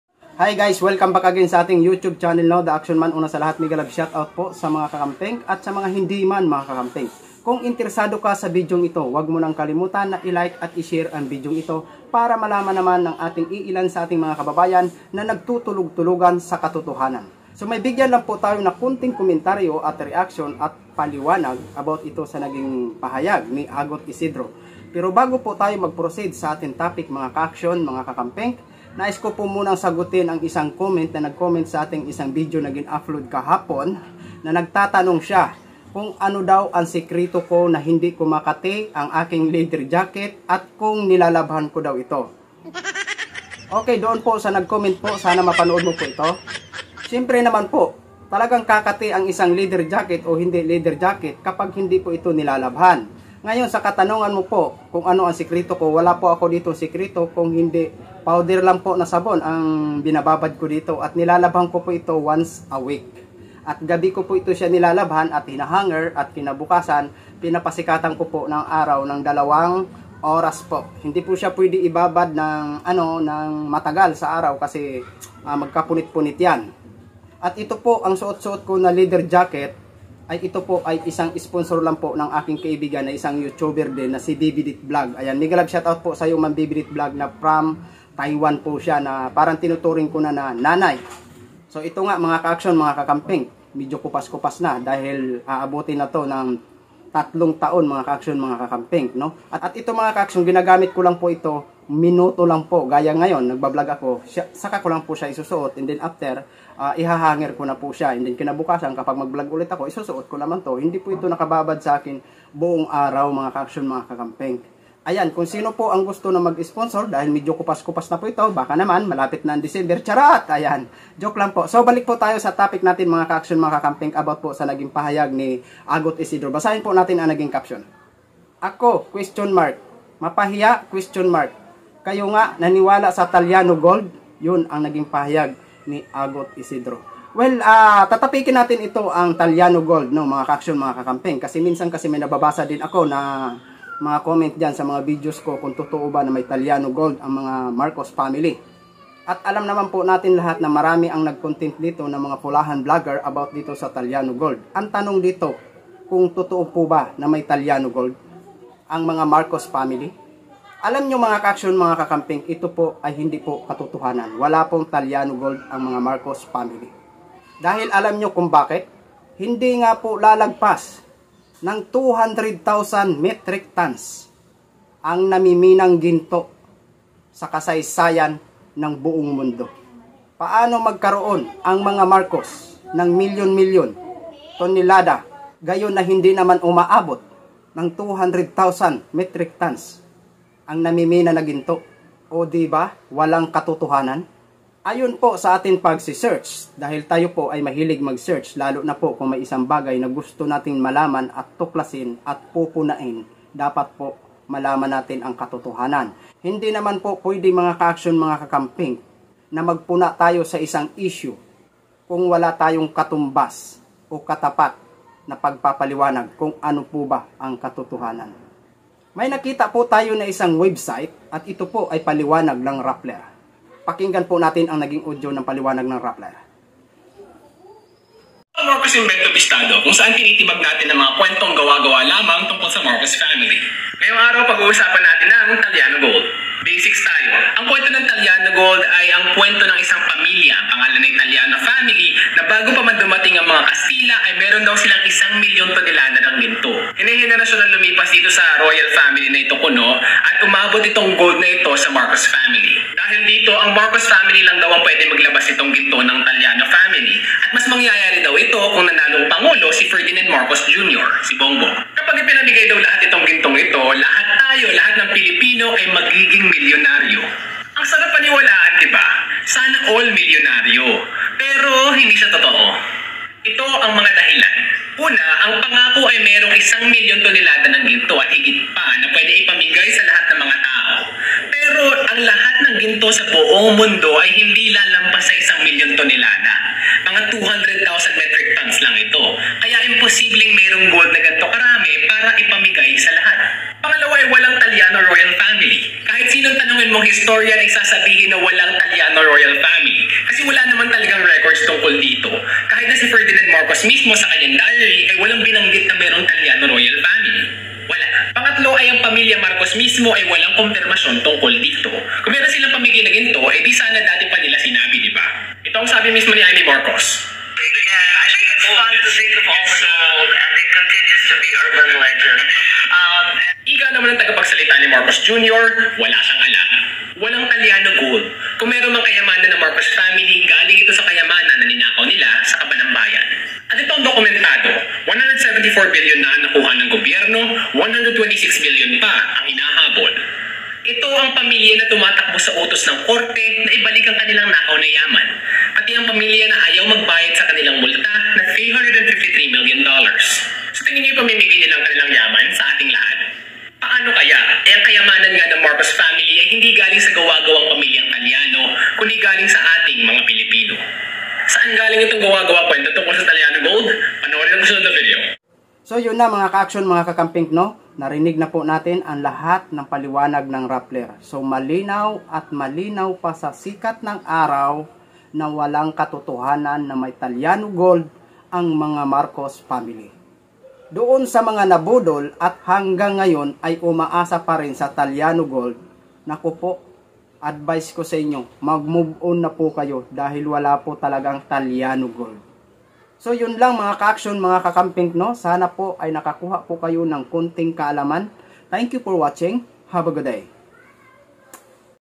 Totoo bang may tallano Gold Ang marcos family? sa mga umaasa para sa inyo ang videong ito.
walang katotohanan na may tallano Gold Ang mga marcos. paano magkaroon ng milyon milyong tonelada Ang mga marcos. eh Hindi nga umabot ng 200k metric tons Ang namimina na ginto sa kasaysayan ng buong mundo.